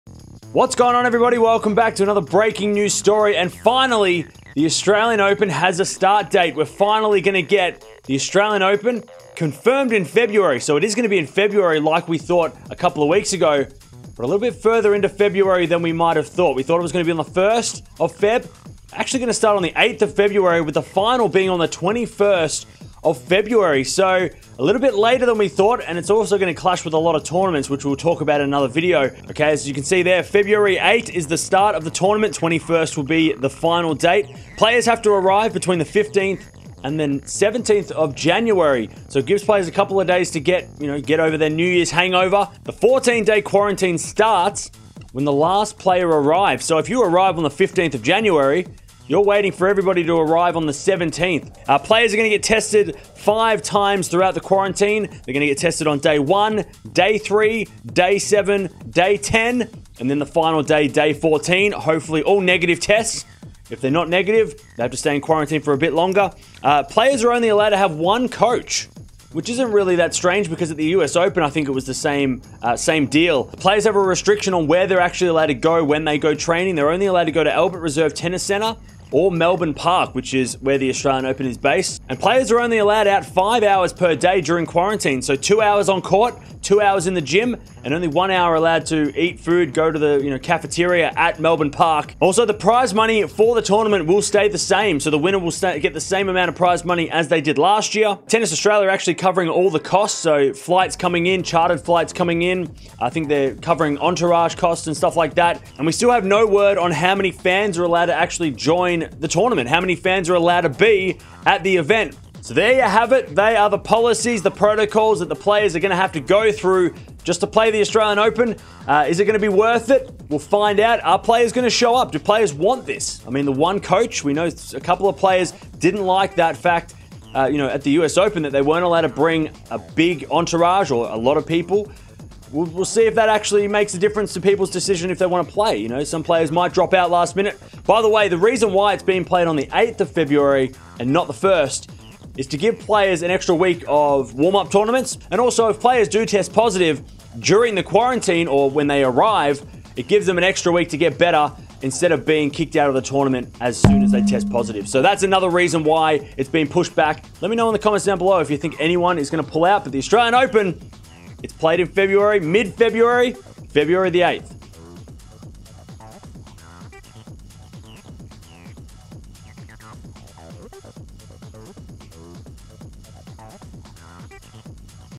Australian Open 2021 CONFIRMED For February | Tennis News
Australian Open 2021 has confirmed to start on February 8th with the final being played on February 21st. Players will have to quarantine for 14 days and will be able to train during this time.